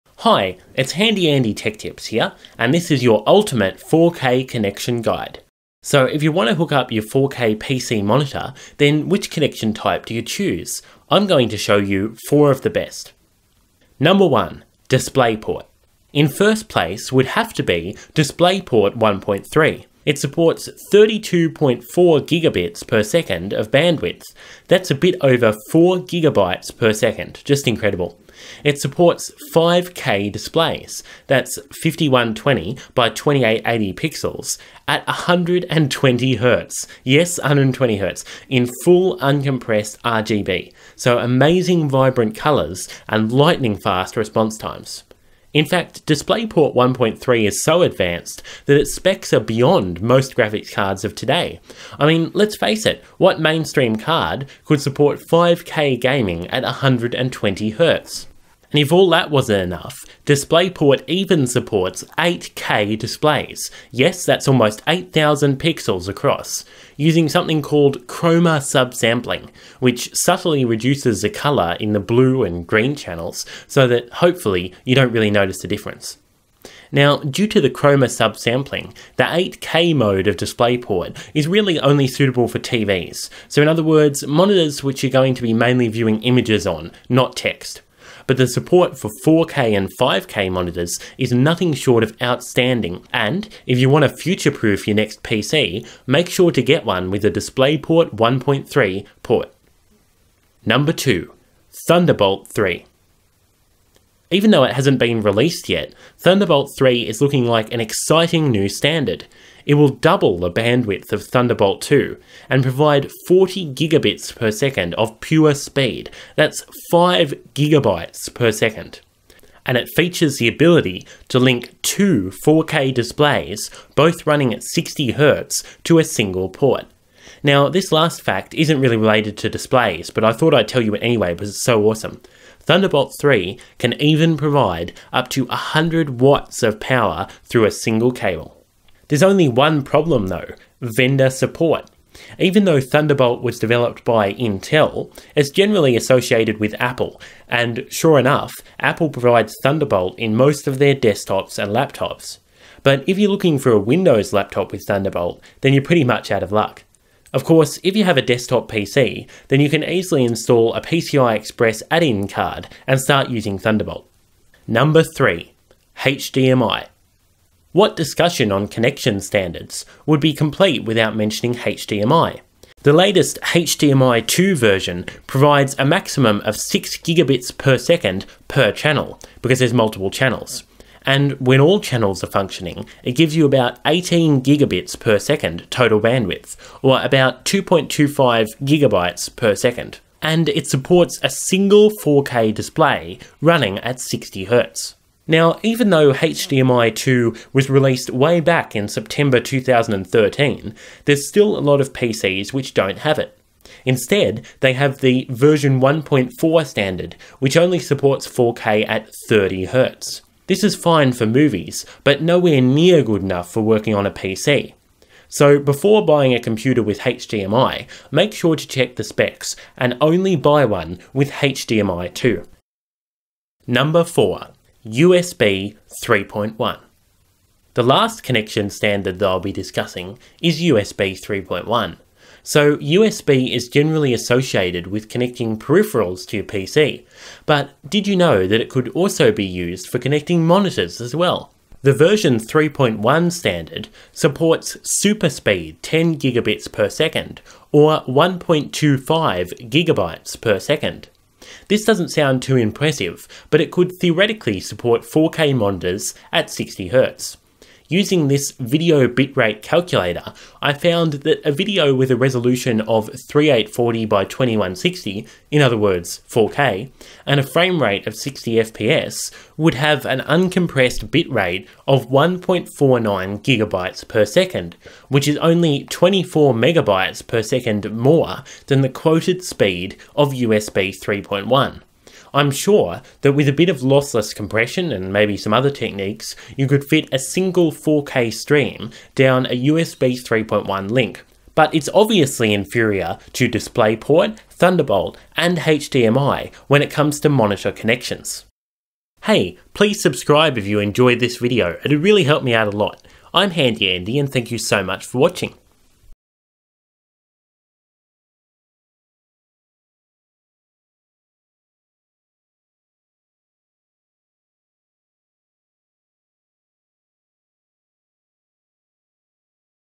4K/60Hz connection SHOWDOWN: DisplayPort 1.3 vs Thunderbolt 3 vs HDMI 2.0 vs USB 3.1
Which connection type is best for Ultra HD, 4K PC monitors? I'll compare the bandwidth, maximum resolution and maximum frame rate (60FPS/60Hz/30Hz/30FPS) of various cables and standards, including DisplayPort 1.3, Thunderbolt 3, HDMI 2 and USB 3.1, and you'll find out which one comes out on top.

PARTIAL VIDEO TRANSCRIPT:
Hi, it's HandyAndy Tech Tips here, and this is your ultimate 4K connection guide. So if you want to hook up
your 4K PC monitor, then which connection type do you choose? I'm going to show you four of the best.

#1 DISPLAYPORT - In first place would have to be DisplayPort 1.3. Introduced in September 2014, it supports
32.4Gbit/s of bandwidth: that's a bit over 4 gigabytes per second! It supports 5K displays: that's 5120 x
2880 pixels at 120Hz in full uncompressed RGB, so amazing vibrant colours and lightning fast response times.
In fact DisplayPort 1.3 is so advanced that its specs are beyond most graphics cards of today: I mean let's
face it, what mainstream card could support 5K gaming at 120Hz? DisplayPort even supports 8K displays (yes,
that's almost 8000 pixels across) using something called chroma subsampling, which
subtly reduces the colour in the blue and green channels so that, hopefuly, you don't notice the difference.

Now due to the chroma subsampling, the 8K mode of DisplayPort is really only suitable for TVs, so in other
words monitors that you're going to be mainly viewing images on, not text. But the support for 4K and 5K
monitors is nothing short of outstanding, and, if you want to future proof your next PC, make sure to
get one with a DisplayPort 1.3 port.

#2 THUNDERBOLT 3 -  Even though it hasn't even been released yet, Thunderbolt 3 is looking like an exciting new
standard. It will double the bandwidth of Thunderbolt 2, and provide 40Gbit/s of pure speed - that's 5 gigabytes
per second. It will include support for many protocols, including PCI Express 3, and features the ability to link
two 4K displays, both running at 60Hz, to a single port. This is not really related to displays,
but Thunderbolt 3 can even provide up to 100W of power through a single cable.

There's only one problem though: vendor support. Even though Thunderbolt was developed by Intel, it's generally
associated with Apple, and, sure enough, Apple provides Thunderbolt in most of their desktops and
laptops. But if you're looking for a Windows laptop with Thunderbolt, then you're pretty much out of luck.
Of course, if you have a desktop PC, then you can easily install a PCI Express add-in card and start using
Thunderbolt.

#3 HDMI What discussion on connection standards would be complete without mentioning HDMI? This has been a mainstay of
home theatres since 2003, and it's also very useful for connecting PC displays. The latest HDMI 2 version
provides a maximum of 6Gbit/s per second per channel, which, with all channels functioning, gives you about 18Gbit/s of
total bandwidth, or about 2.25 gigabytes per second. It supports a single 4K display at 60Hz.

Now even though HDMI 2 was released way back in September 2013, there's still a lot of PCs which don't have it. Instead,
they have the version 1.4 standard, which only supports 4K at 30Hz. This is fine for movies, but nowhere near good enough
for working on a PC. So before buying a computer with HDMI, make sure to check the specs and only buy one with HDMI 2.

#4 USB 3.1 - The last connection standard that I'll be discussing is USB 3.1. So USB is generally associated with connecting
peripherals to your PC, but did you know it can be used for connecting monitors as well? The version 3.1 standard supports
SuperSpeed 10Gbit/s, or 1.25 gigabytes per second. This doesn't sound too impressive, but it could theoretically support
4K monitors at 60Hz. Using a video bitrate calculator, I found that a video with a resolution of 3840 x
2160 and a framerate of 60FPS, would have an uncompressed bitrate of 1.49 gigabytes per second, which is only 24 megabytes per
second more than the quoted speed of USB 3.1. I'm sure that, with a bit of lossless compression and other techniques,
you could fit a single 4K stream down a USB 3.1 link. But it's obviously inferior to DisplayPort, Thunderbolt and HDMI
when it comes to monitor connections.

4K connection SHOWDOWN: DisplayPort 1.3 vs Thunderbolt 3 vs HDMI 2 vs USB 3.1